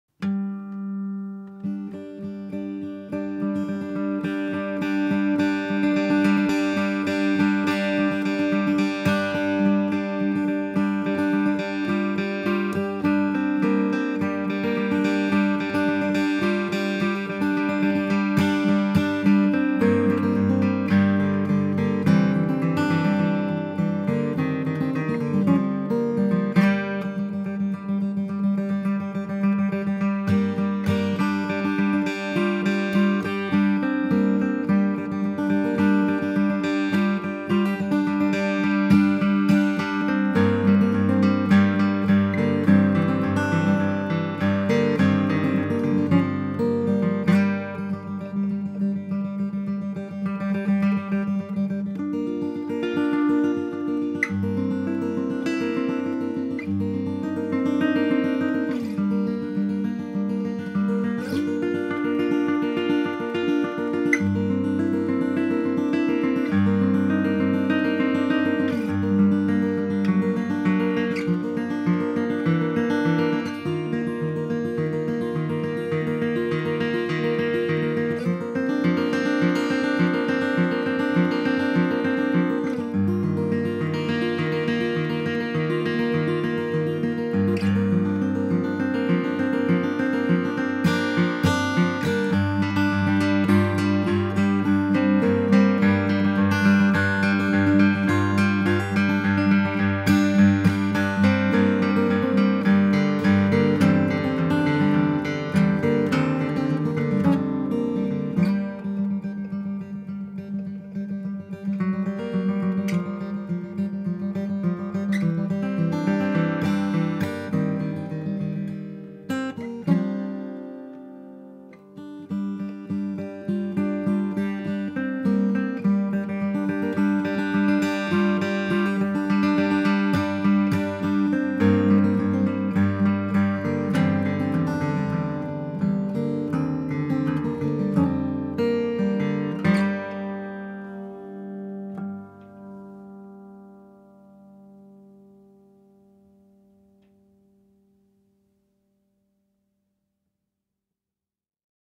McAlister Concert Model Acoustic Guitar Played By Stuart Ryan (Part Two)
This is a gorgeous new arrival in our Pre-Owned section, fine hand crafted guitars by Roy McAlister out in Washington, DC.

The McAlister Concert model takes its inspiration from a Martin OM of the early 30’s. This model features vintage style scalloped bracing and light construction, paired with McAlister contemporary design to create an incredibly versatile guitar.
Despite possessing the volume of a dreadnought, there remains to be exceptional string balance and sophisticated overtone complexity.

It's not surprise that this is the chosen guitar of many incredible musicians, such as; David Crosby, Graham Nash and Jackson Browne.